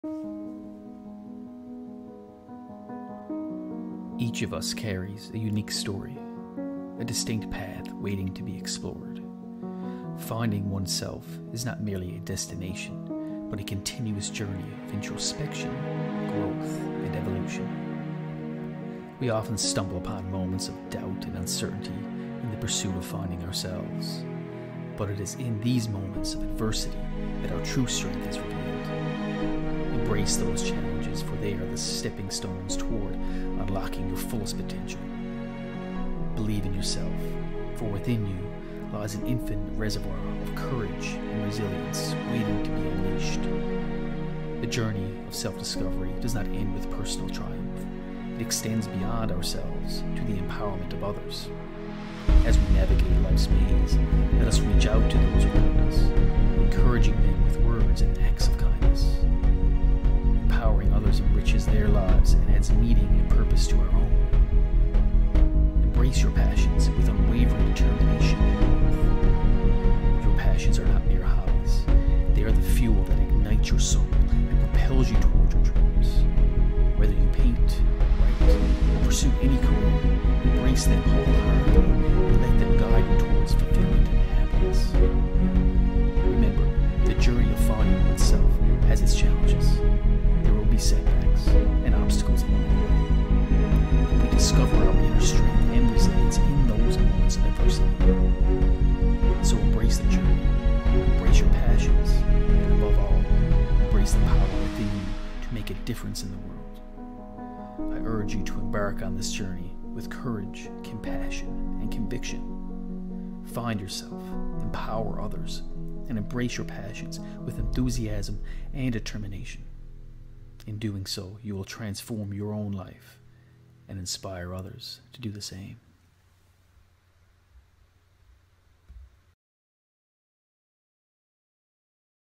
Motivational Speech | Discover Your Power: A Journey of Self-Exploration & Empowerment
Each of us carries a unique story and a path filled with moments of growth and evolution. Embrace challenges as opportunities for growth, and believe in your inner strength and resilience.

Extend your journey beyond yourself by empowering others with kindness and encouragement. Embrace your passions with determination, as they guide you towards fulfillment and happiness.

Embrace this journey with courage, compassion, and conviction. Find yourself, empower others, and inspire change.